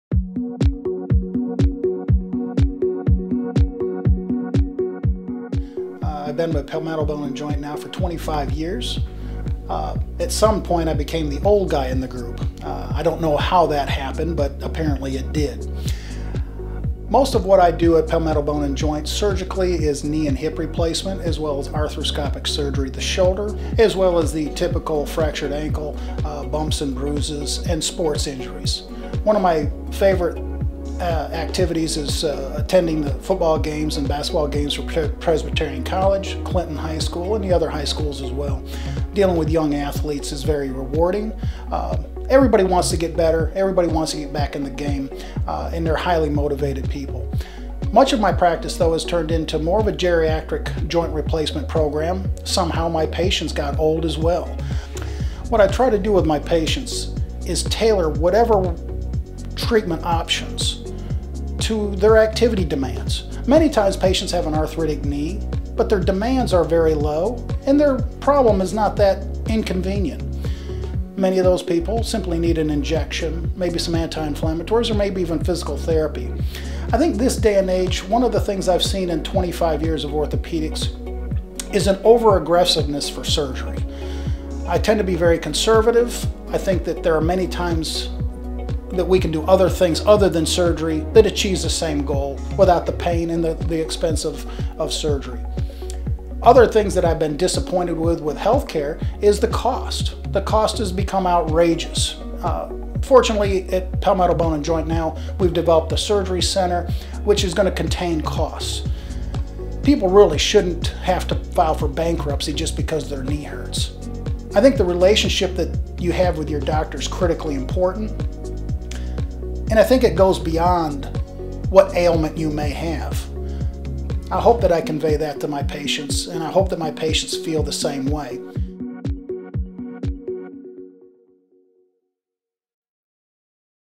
Dr Phillip Milner with Palmetto Bone and Joint
Dr. Phillip Milner with Palmetto Bone and Joint discusses his practice of orthopaedic surgery, care for college and high-school athletes, and cost savings that the Chapin Orthopedic Surgery Center can have for patients.